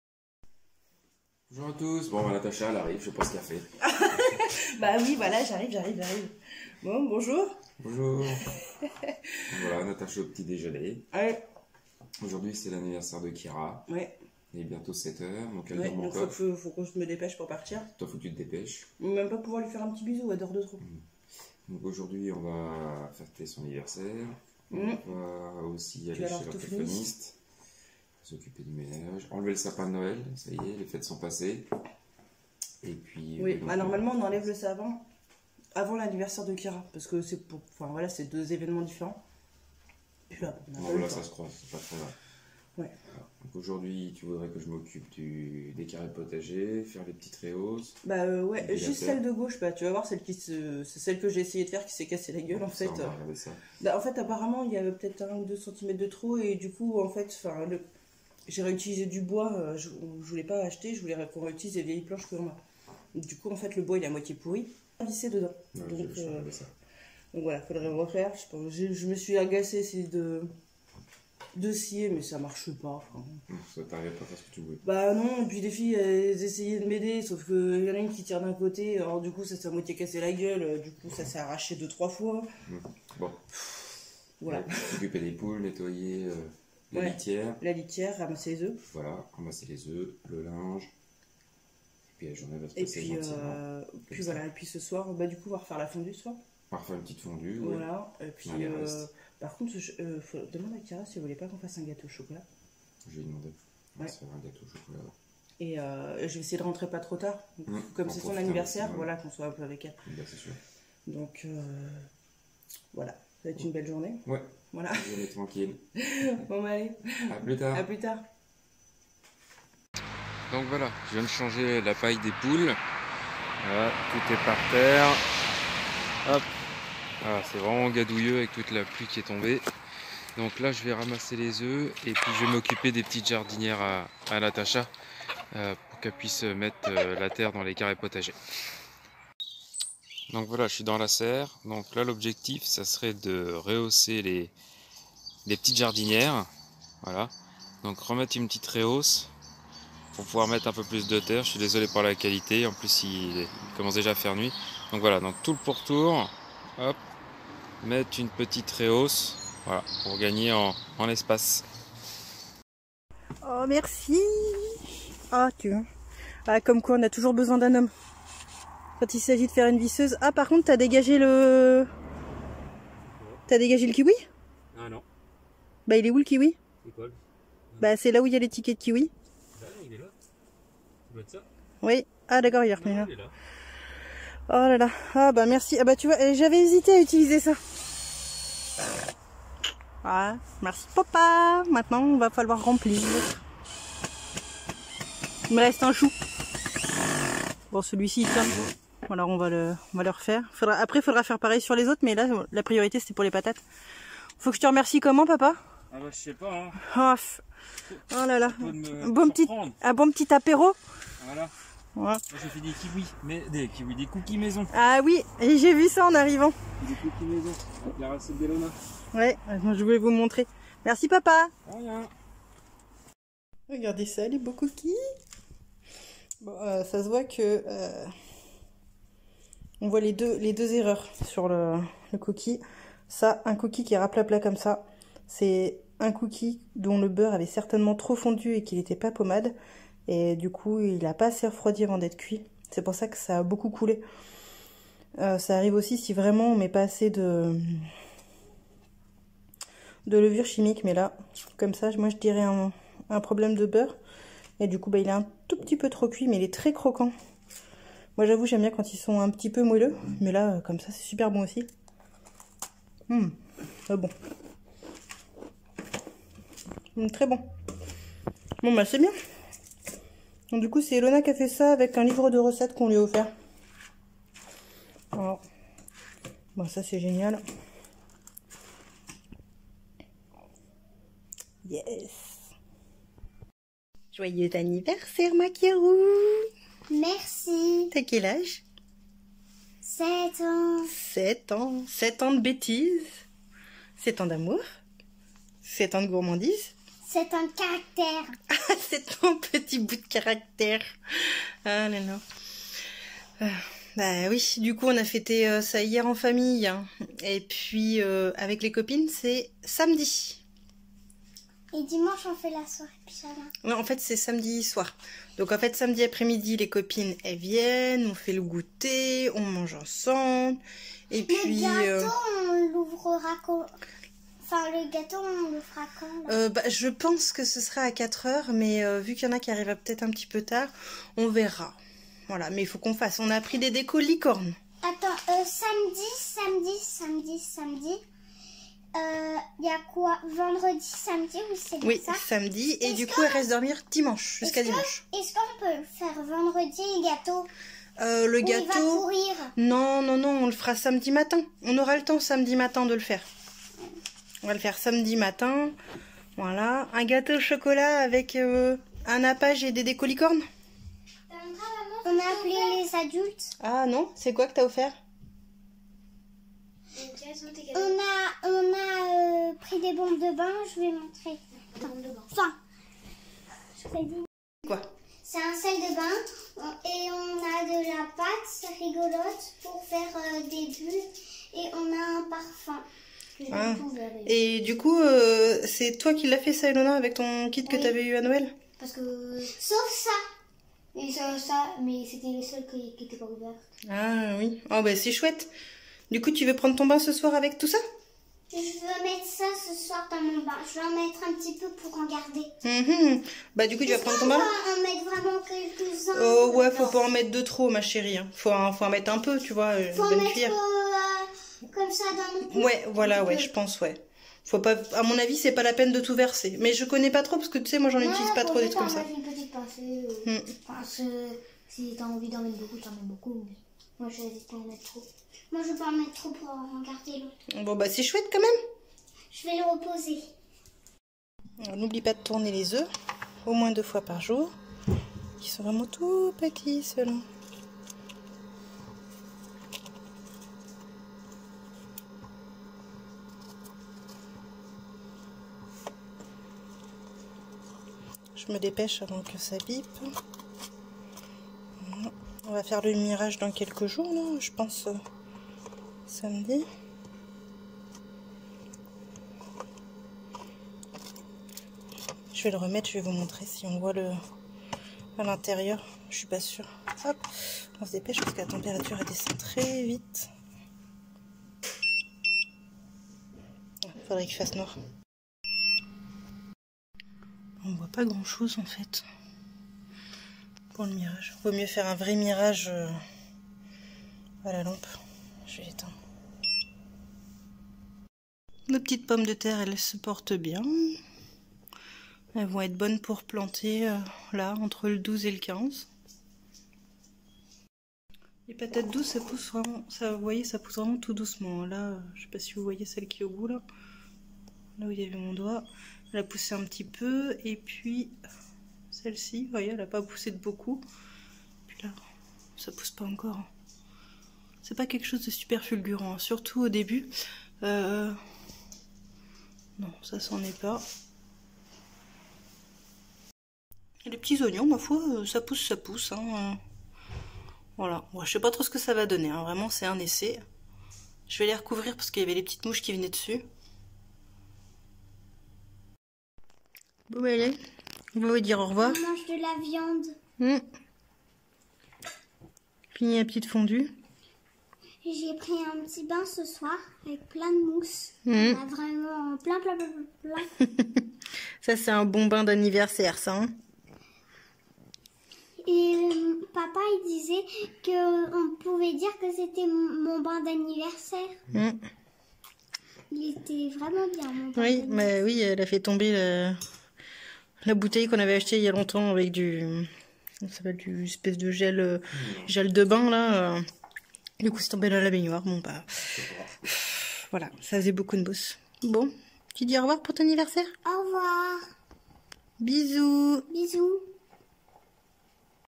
9 janvier 2023 - Anniversaire de Kiara et travail dans la serre
Aujourd'hui c'est l'anniversaire de Kiara, un repas sympa ce soir mais avant il ya toute la journée, les rendez vous, le CNED, le poulailler et la rehausse d'un carre dans la serre

Notre guide zéro déchet tout simple :